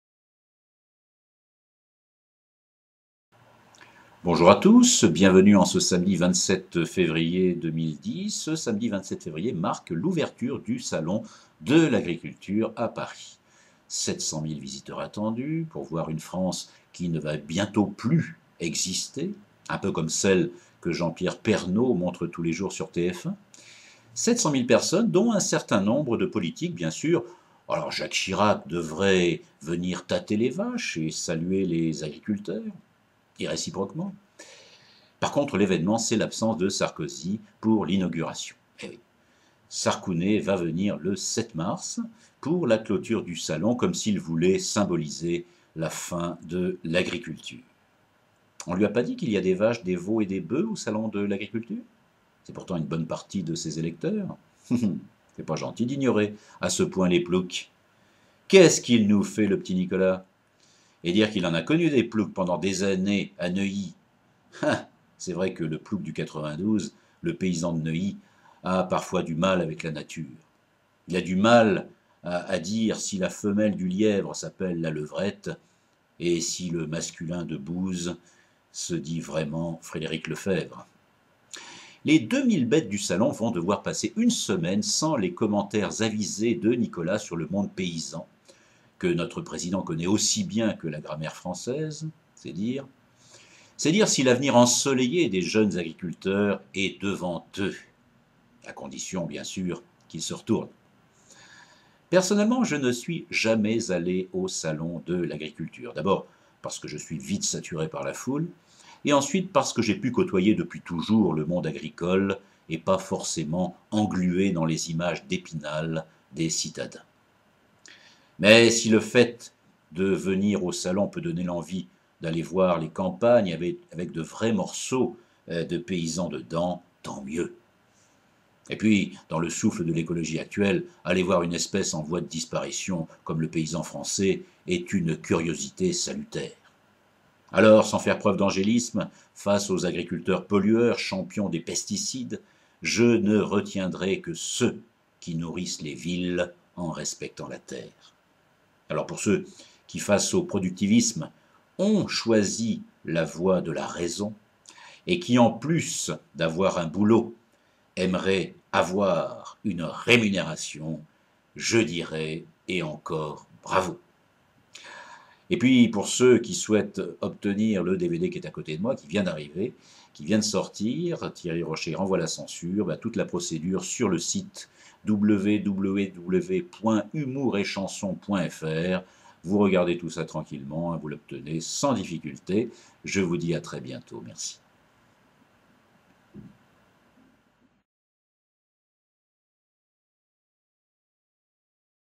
Thierry Rocher renvoie la censure: Salon de l'Agriculture
Le Salon de l'Agriculture s'ouvre sans Sarkozy. Mais des veaux et des boeufs pour rappeler ses amis!